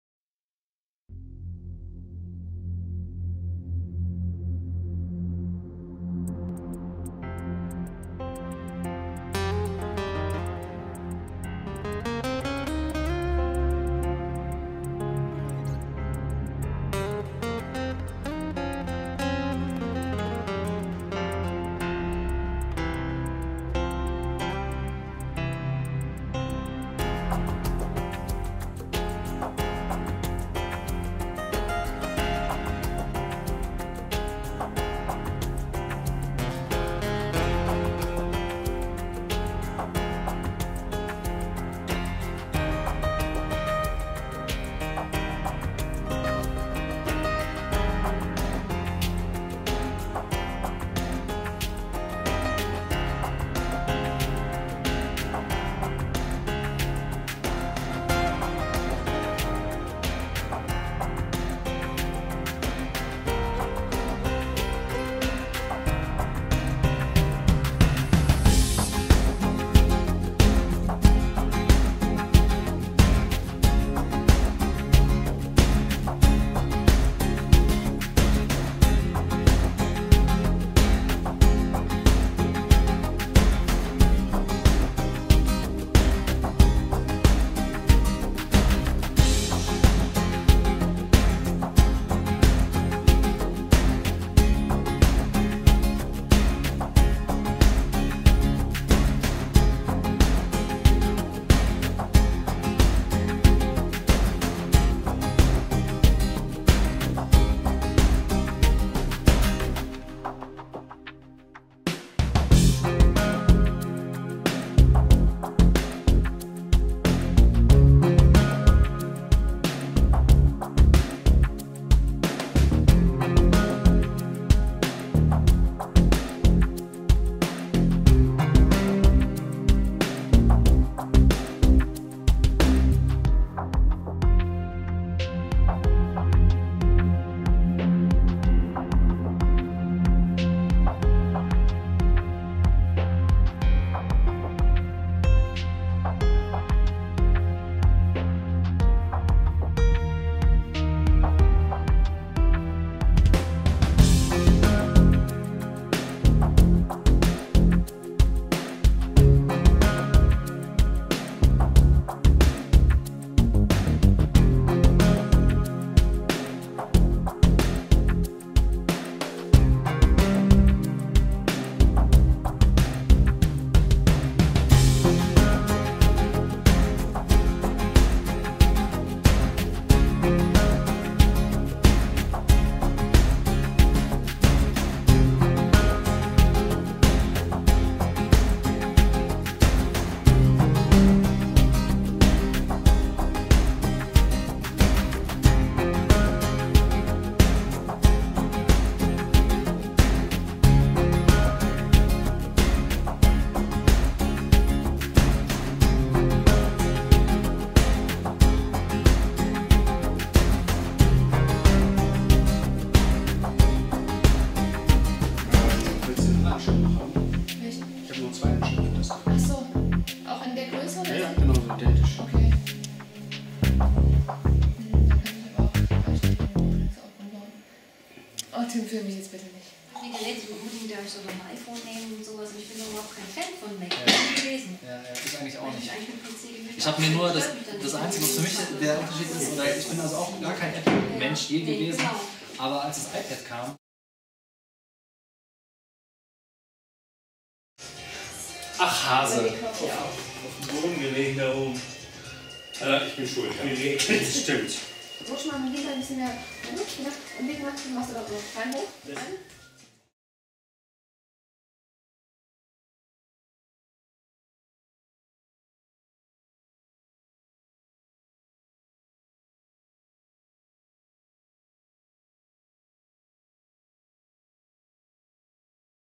Pacator 04 - Fotoshooting
PACATOR Fotoshooting mit Marija, Rames, Tim, Melanie, Frank und Sabine.

Vielen Dank an Melanie Niesik vom Fotostudio Erzhausen für die tolle Unterstützung!